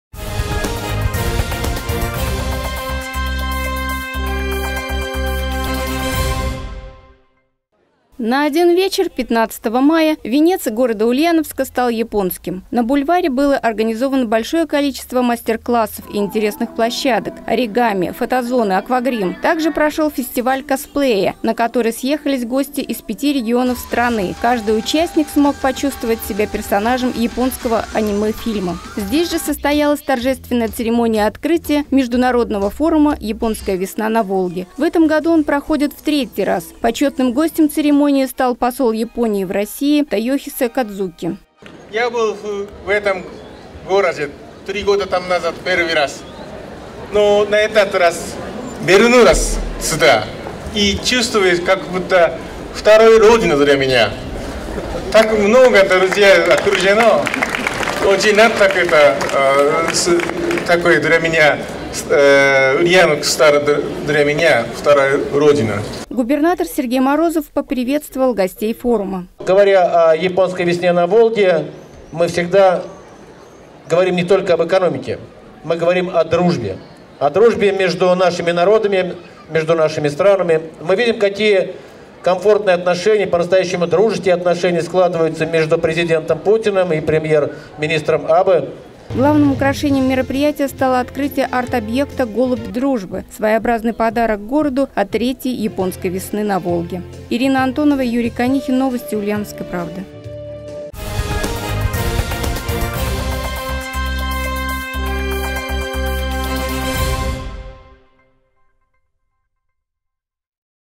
Япония подарила Ульяновску «Голубя дружбы»
На один вечер 15 мая Венец города Ульяновска стал японским. На бульваре было организовано большое количество мастер-классов и интересных площадок: оригами, фотозоны, аквагрим.